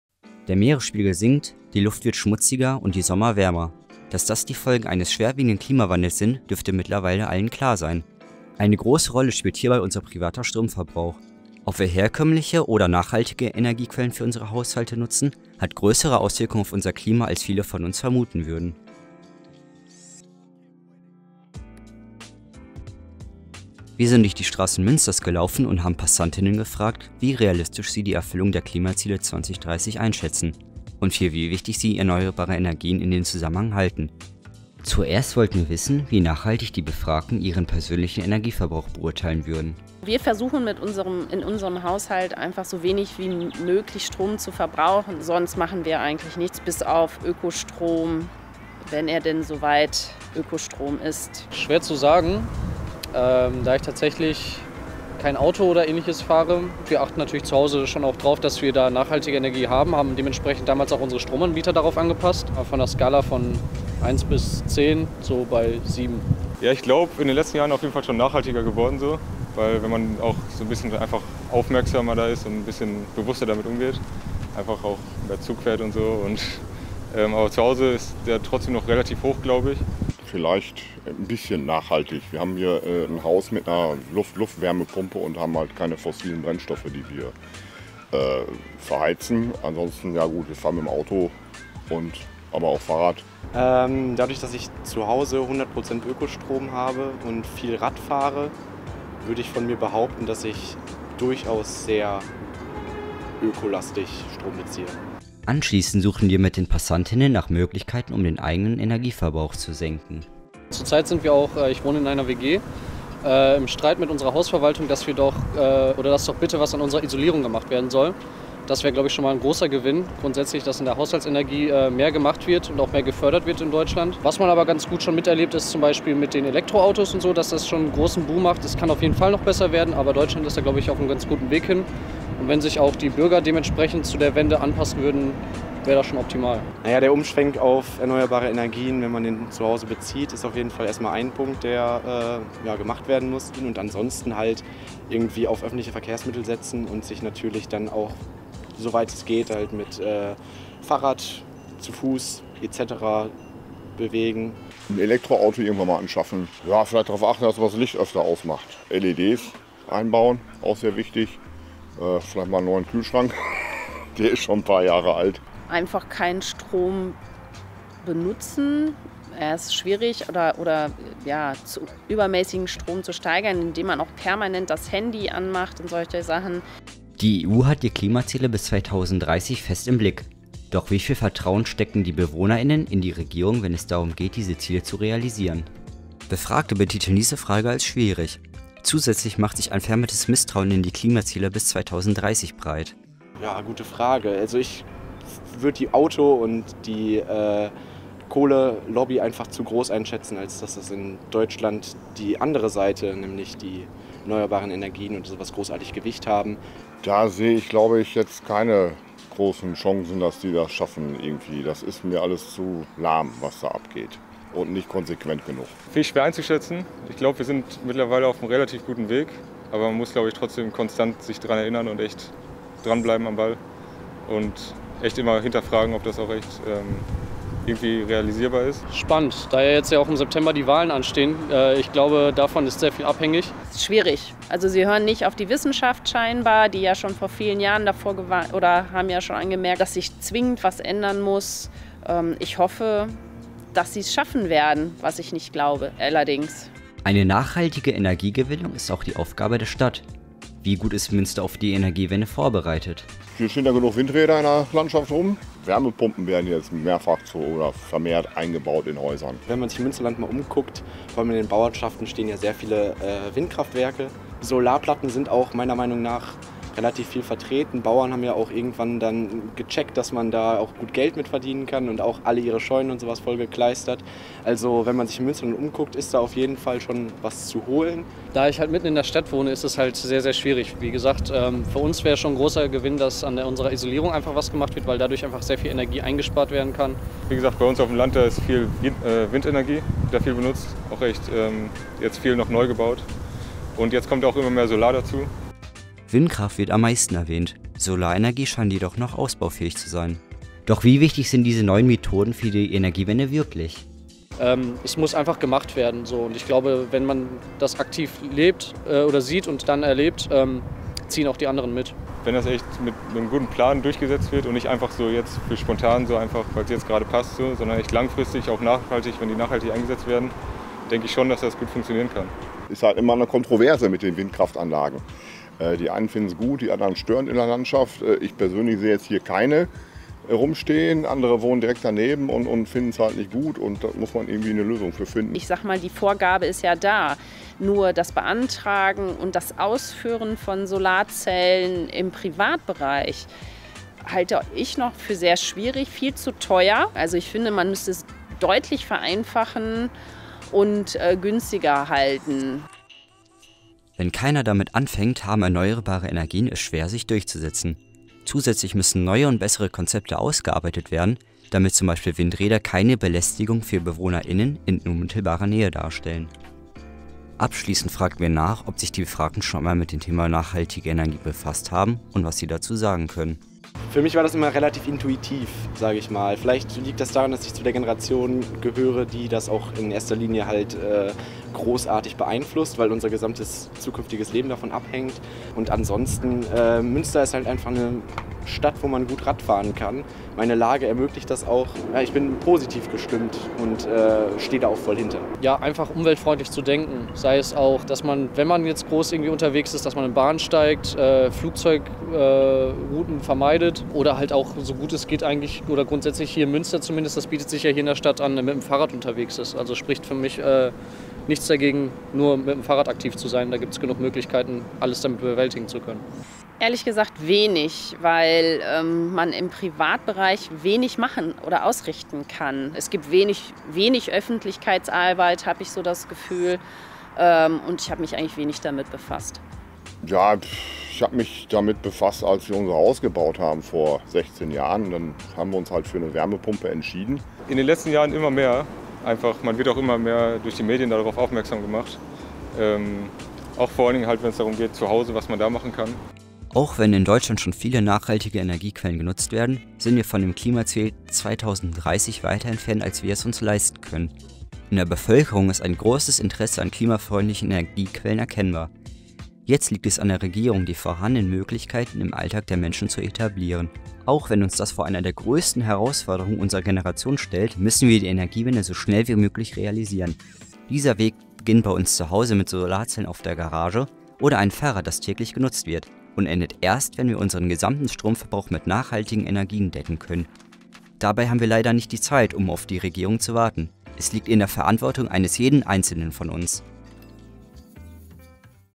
Klimaziele - Der Planet in deinen Händen | Ostviertel
Eines der insgesamt 17 Ziele für nachhaltige Entwicklung, die bis zum Jahr 2030 auf globaler Ebene erreicht werden sollen, ist die Gewährleistung des Zugangs zu erschwinglicher, zuverlässiger, nachhaltiger und moderner Energie für alle. Wir haben Münsteraner gefragt, was sie tun, um ihren eigenen Energieverbrauch nachhaltig zu gestalten.

Im Monat Juni der Projektaktion “Ein Planet – eine Freundschaft – eine (E)mission” setzen sich junge Menschen aus den Partnerstädten Orléans und  Münster mit dem Thema "Energie" aus lokaler Perspektive auseinander.

 Luca Eilers, Jan Lichtenstein, Alexander Jacob